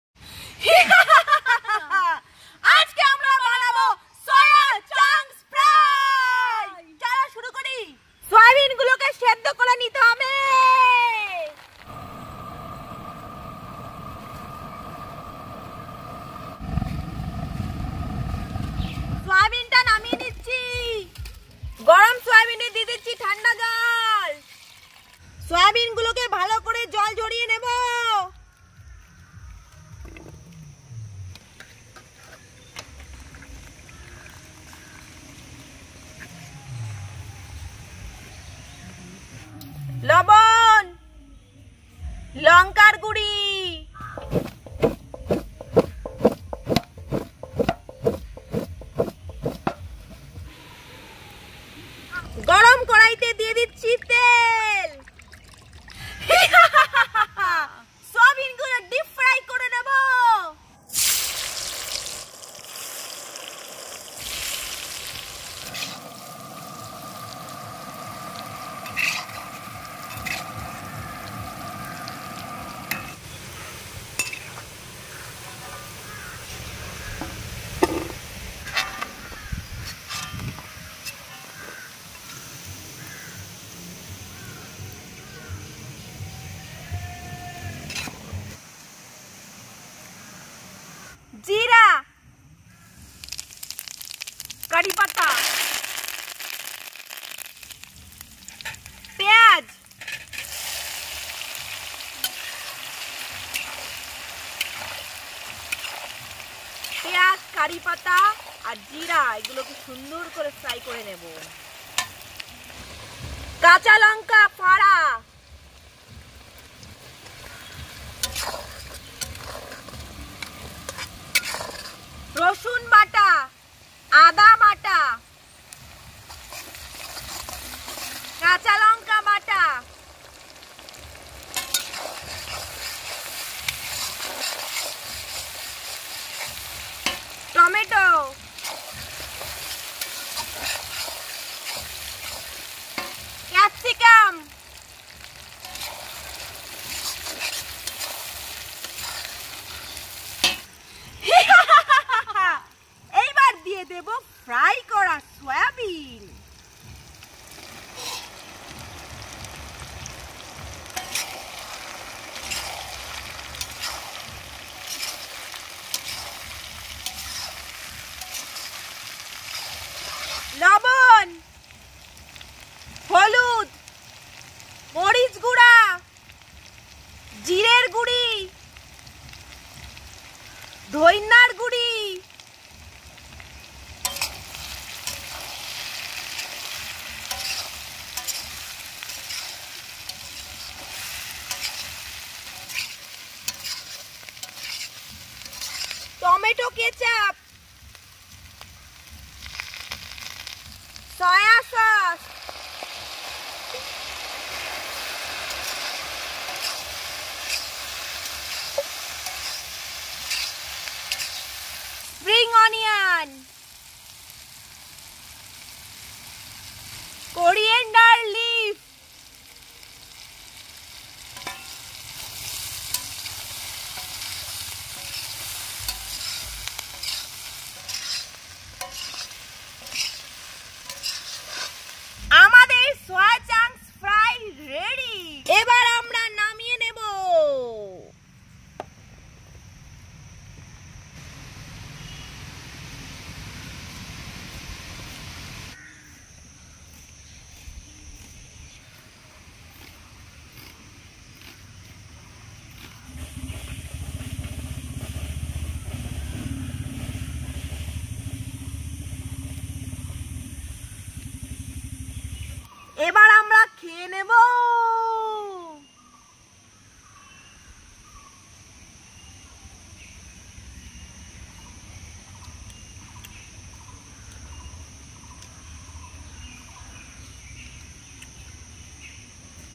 Easy Snacks Recipe - Soya Chunks Fry | Spicy & Full Of Protein Snacks Recipe in Bengali
Hey guys I am Anindita & today I'm back with an exciting snacks recipe. Soya chunks is source of protein & this recipe tastes amazing. you will definately enjoy the cooking.
Soyabean Recipe in Bengali
Easy Soyabean Recipes
Soya Chunks Fry
Evening Snacks Recipe
Easy Snacks Recipe in Bengali